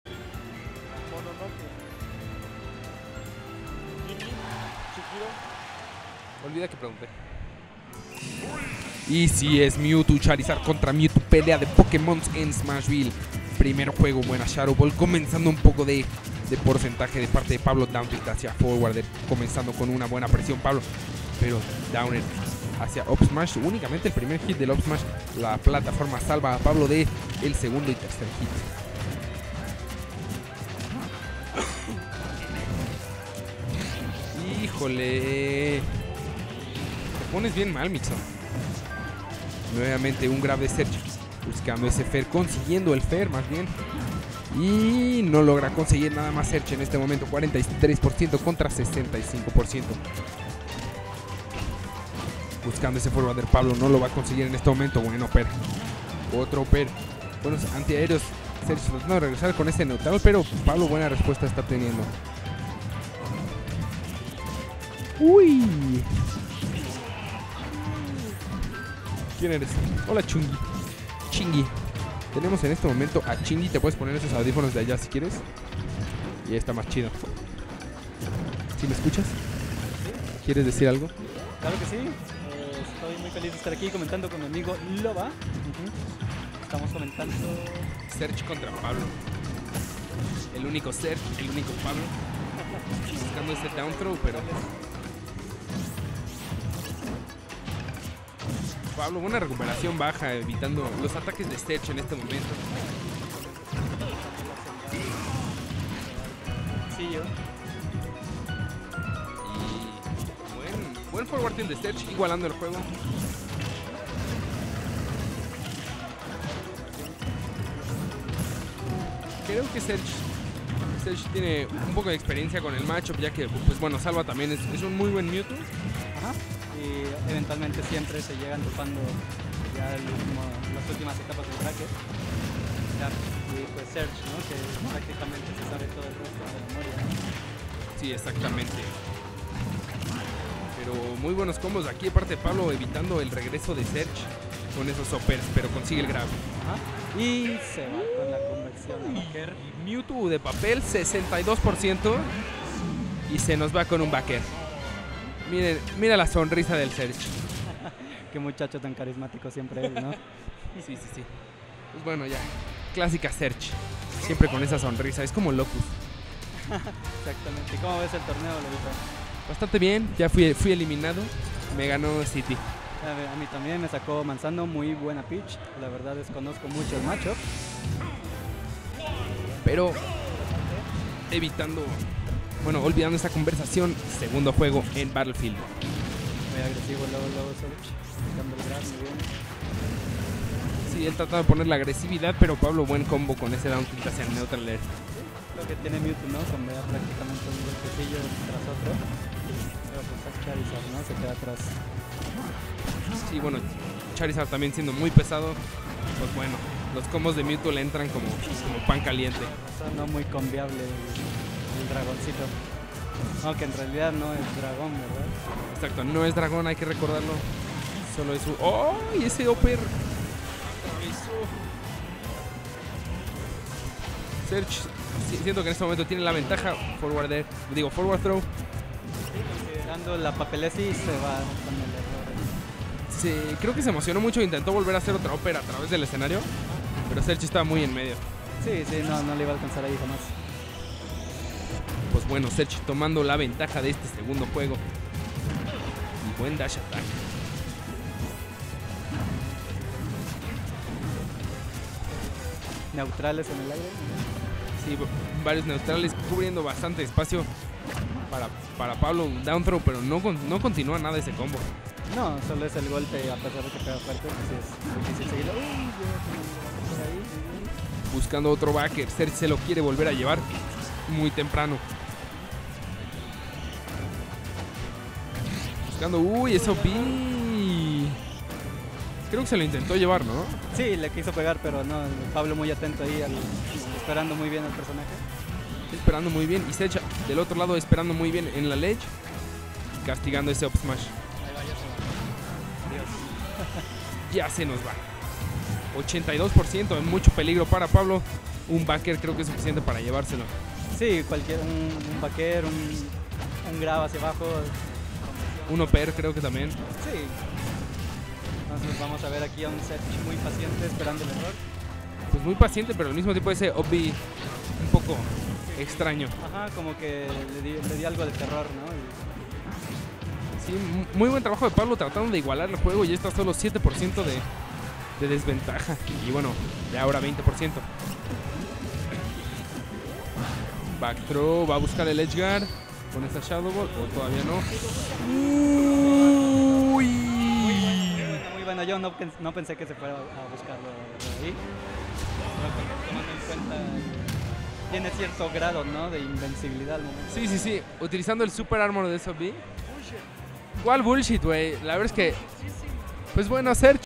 TheMiddle #23 - SF | Serge (Chari) Vs. Pablo (Mewtwo) - Winners Semis - Singles
The Middle #23, 26/07/2017, CDMX.